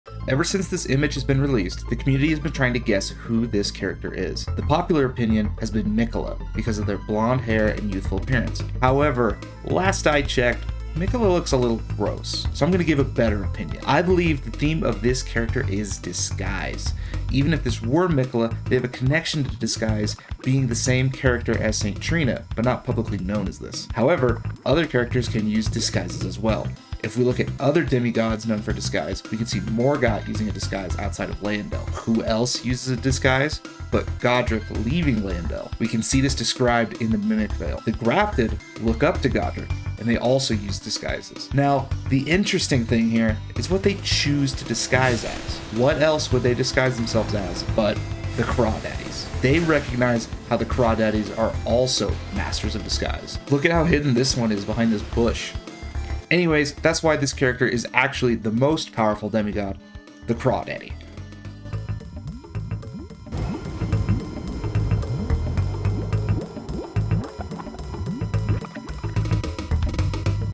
Shadow of the Erdtree Character Prediction
It has to be Miquella right? It couldn't be anything else, right?